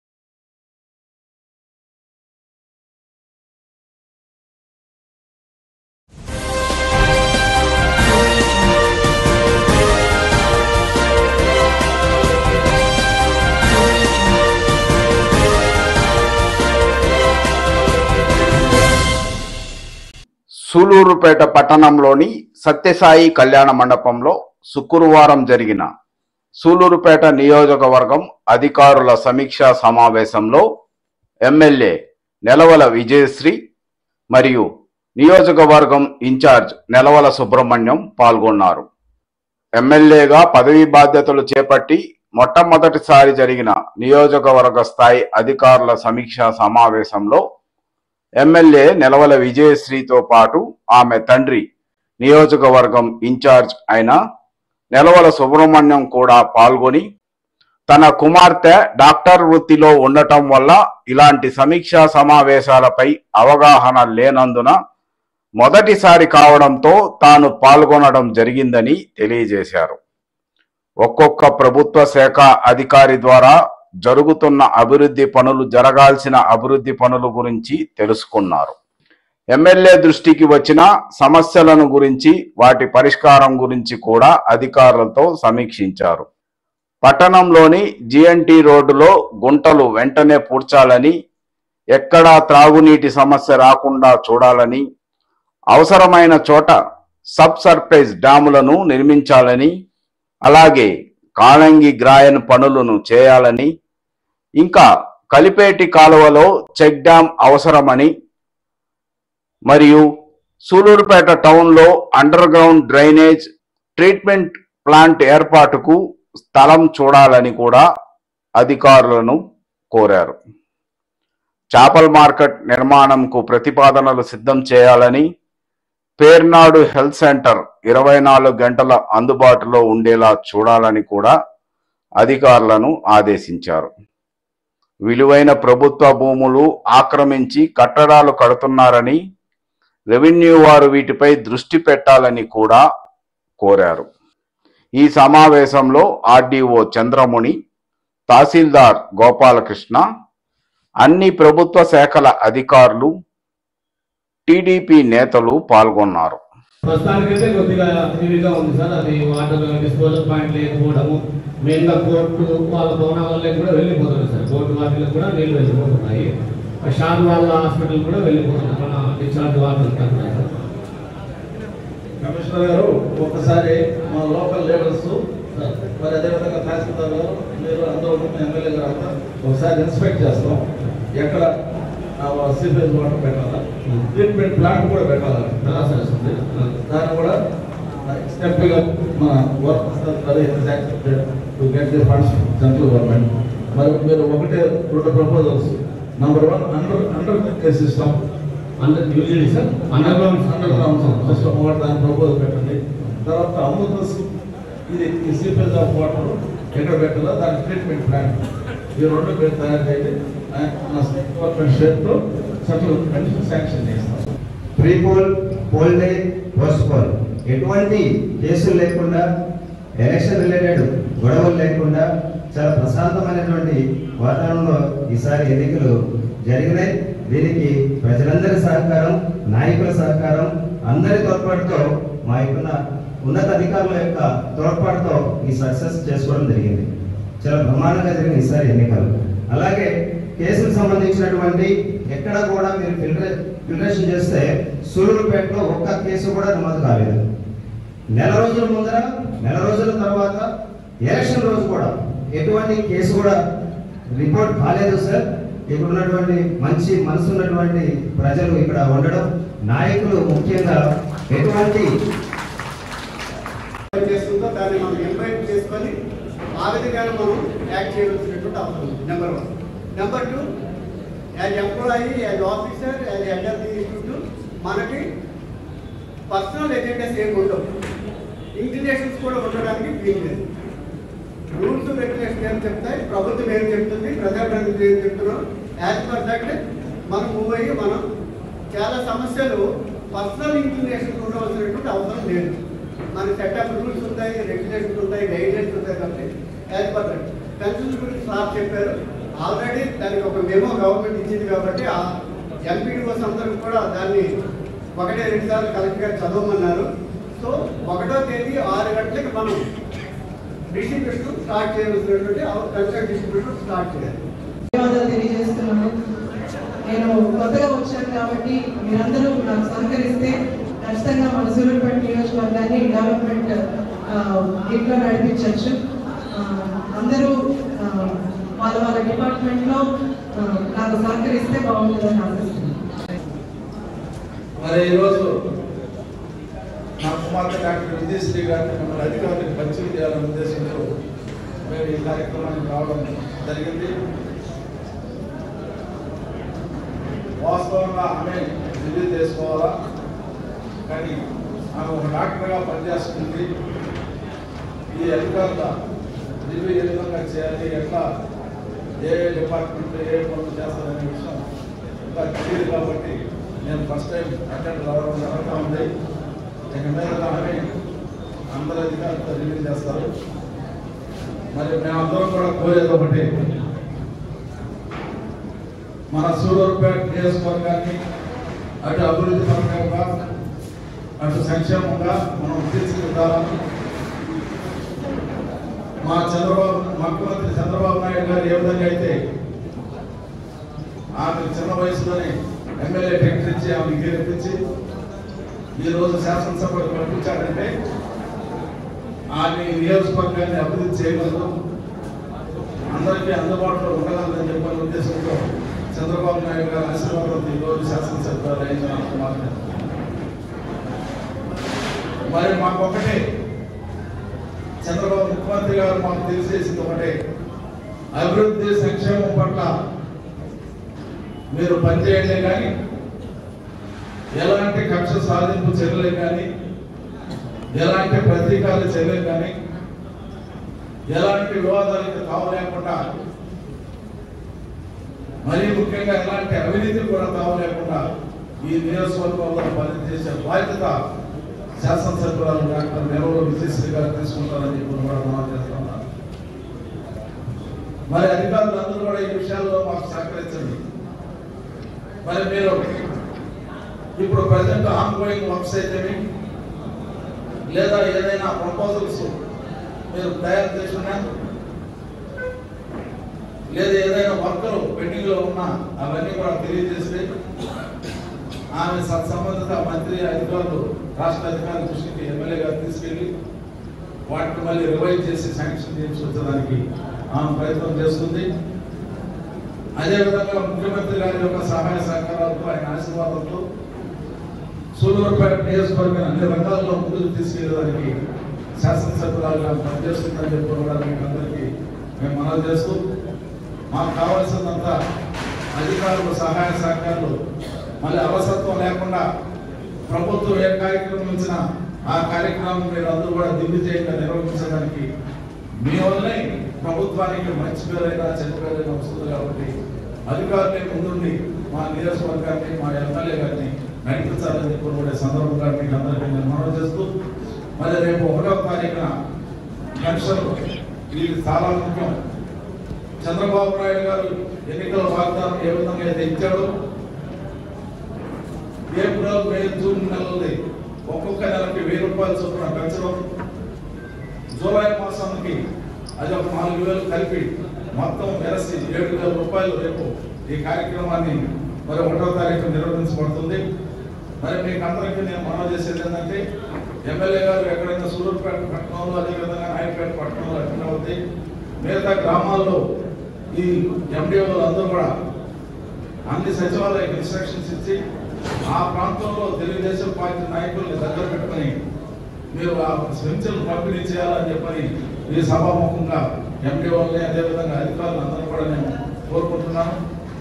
అధికారుల తో MLA సమీక్షా సమావేశం సమస్యల పై ద్రుష్టి
అధికారుల తో MLA
సమీక్షా సమావేశం
సమస్యల పై ద్రుష్టి

సూళ్లూరుపేట లో శుక్రవారం జరిగిన సూళ్లూరుపేట నియోజకవర్గం అధికారుల సమీక్షా సమావేశం లో MLA నెలవల విజయశ్రీ , నియోజకవర్గం ఇంచార్జి నెలవల సుభ్రమణ్యం
పాల్గొన్నారు, MLA గా పదవీ బాధ్యతలు చేపట్టి మొట్ట మొదటి సారి జరిగిన నియోజకవర్గ స్థాయి అధికారుల సమీక్షా సమావేశం లో MLA నెలవల విజయశ్రీ తో పాటు ఆమె తండ్రి నియోజకవర్గం ఇంచార్జి ఐన నెలవల సుబ్రహ్మణ్యం కూడా పాల్గొని తన కుమార్తె డాక్టర్ వృత్తిలో ఉండటం వల్ల ఇలాంటి సమీక్ష సమావేశాలు పై అవగాహన లేనందున మొదటి సారి కావడం తో తాను పాల్గొనడం జరిగిందని తెలియజేసారు, ఒక్కొక్క ప్రభుత్వ శాఖా అధికారి ద్వారా జరుగుతున్న అభివృద్ధి పనులు జరగాల్సిన పనులు గురించి తెలుసుకున్నారు,MLA దృష్టికి వచ్చిన సమస్యలను గురించి వాటి పరిష్కారం గురించి కూడా అధికారులతో సమీక్షించారు, పట్టణం లోని GNT రోడ్డులో గుంటలు వెంటనే పూడ్చాలని ,ఎక్కడా త్రాగునీటి సమస్య రాకుండా చూడాలని,అవసరమైన చోట సబ్ సర్ఫర్స్ డ్యాములు నిర్మించాలని,కాళంగి గ్రాయన్ పనులు చేయాలనీ,కలిపేటి కాలువలో చెక్ డ్యాం అవసరమని, సూళ్లూరుపేట టౌన్ లో అండర్ గ్రౌండ్ డ్రైన్,ట్రీట్మెంట్ ప్లాంట్ ఏర్పాటుకు స్థలం చూడాలని,చేపల మార్కెట్ నిర్మాణం కు ప్రతిపాదనలు సిద్ధం చేయాలనీ ,పెర్నాడు హెల్త్ సెంటర్ 24 గంటలు అందుబాటులో ఉండేలా చూడాలని , విలువైనా ప్రభుత్వ భూములు ఆక్రమించి కట్టడాలు కడుతున్నారని రెవెన్యూ వారు వీటి పై ద్రుష్టి పెట్టాలని ఈ సమీక్షా సమావేశం అధికారులలో సమీక్షించడం జరిగింది, ఈ సమావేశం లో RDO చంద్రముని ,తాసీల్ధార్ గోపాలకృష్ణ ,అన్ని ప్రభుత్వ శాఖల అధికారులు ,టిడిపి నేతలు పాల్గొన్నారు.